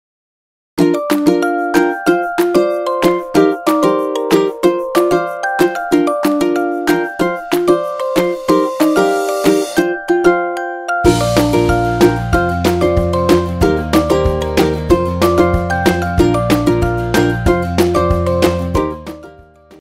Πώς θα καταλάβετε ότι το παιδί σας βρίσκεται σε αναπτυξιακό στάδιο; | NOVALAC
Tα μωράκια μεγαλώνουν και αναπτύσσονται ραγδαία. Δες το video για πώς αλλάζει η συμπεριφορά του μωρού σου.